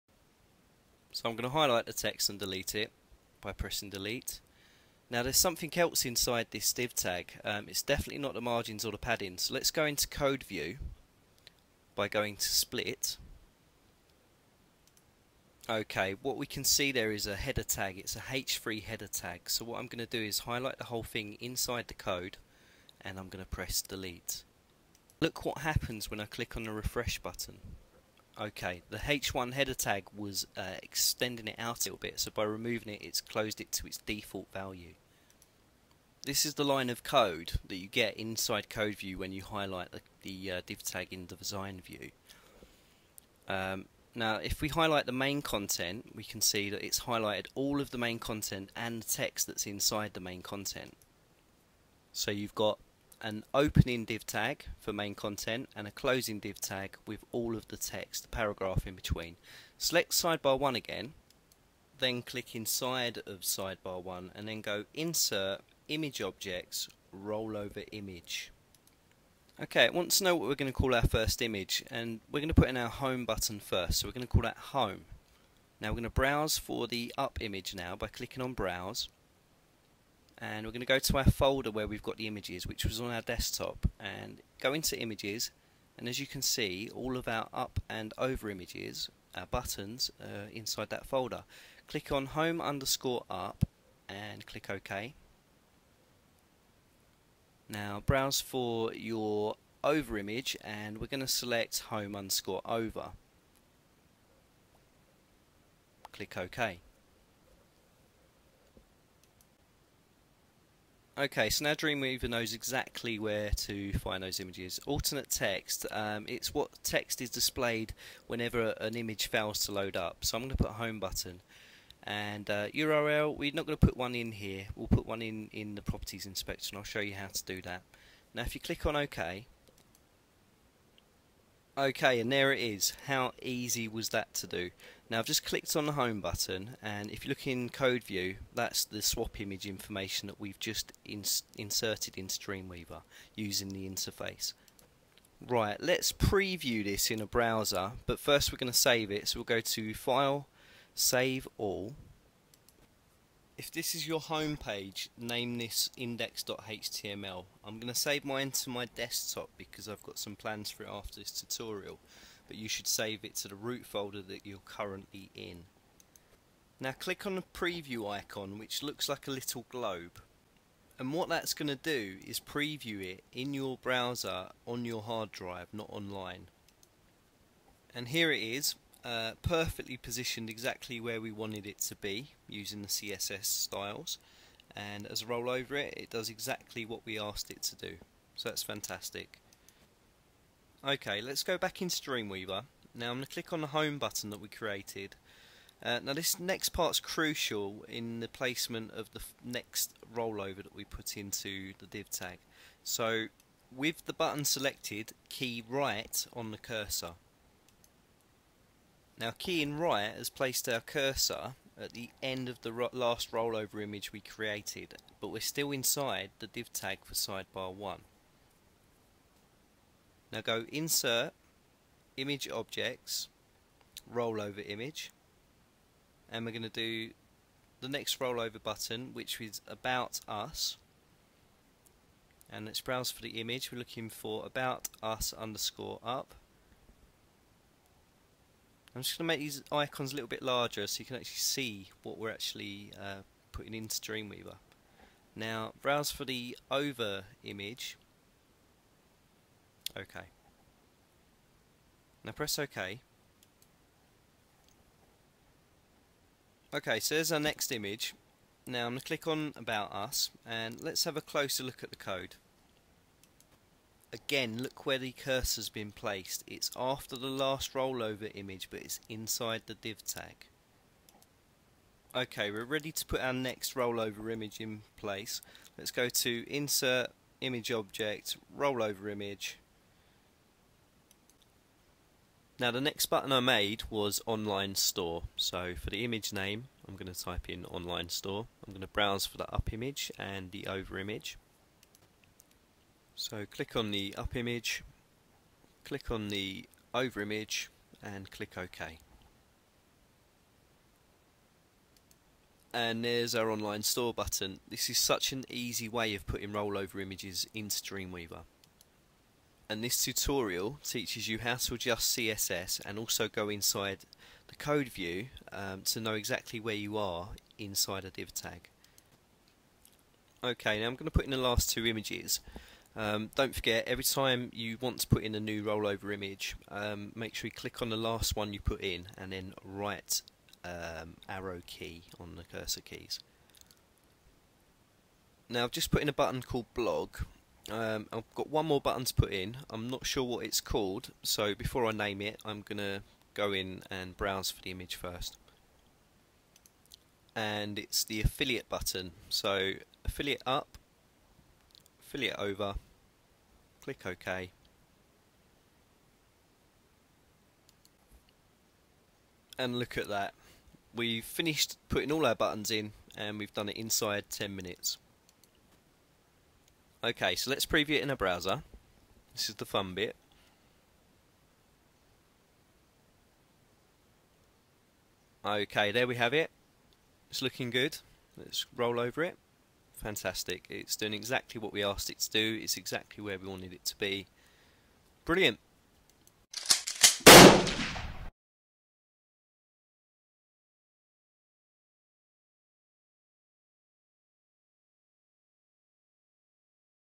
Insert Rollover Images into Dreamweaver part 2
we will be inserting rollover images into a dreamweaver template.  we will learn to adjust the CSS style sheet to fit the rollover images, and we will also learn how the designer view and code view together will help you to navigate through your site with ease.

(website available from mid June.